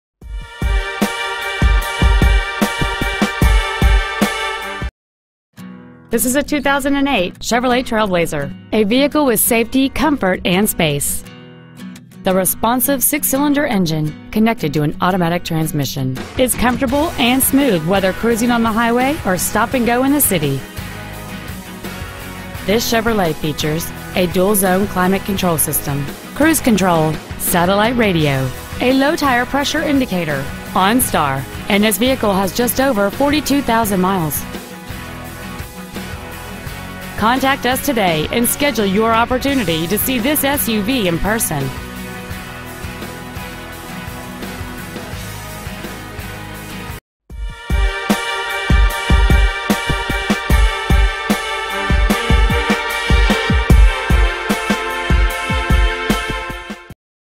Used 2008 Chevrolet TrailBlazer Bridgeview IL
Year: 2008
 Make: Chevrolet
 Model: TrailBlazer
 Engine: Gas I6 4.2L/254
 Trans.: Automatic
 Exterior: Silverstone Metallic
 Miles: 42,999
 Interior: Gray

 2008 Chevrolet TrailBlazer Bridgeview IL